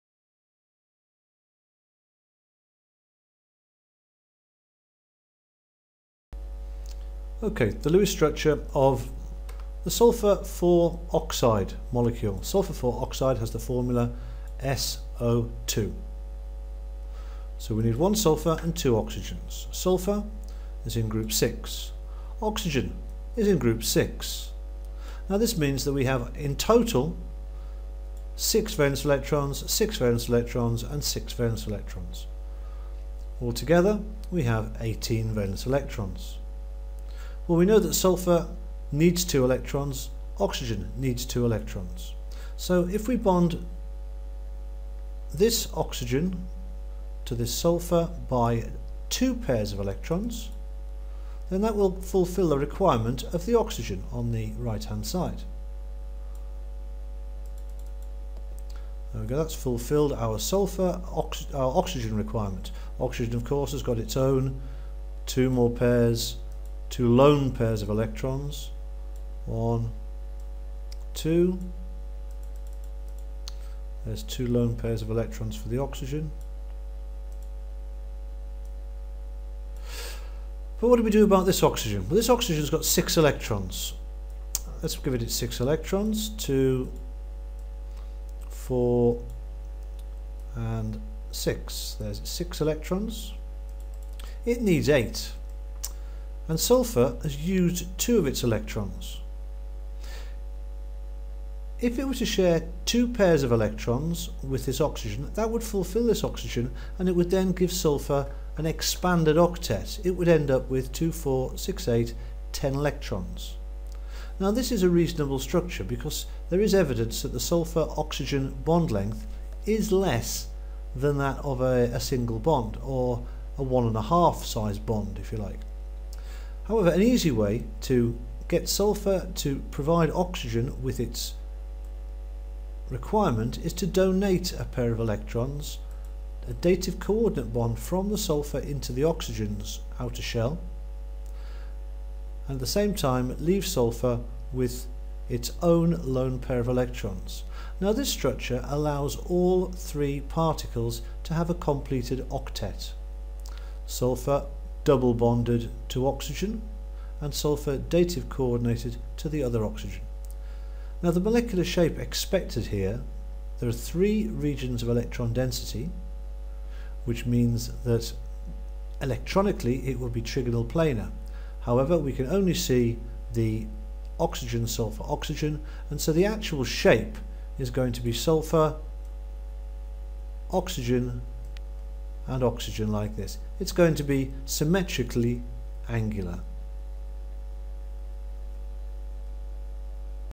The Lewis structure of sulphur(IV) oxide
The Lewis structure of sulphur(IV) oxide showing the use of VSEPR theory to arrive at the molecular shape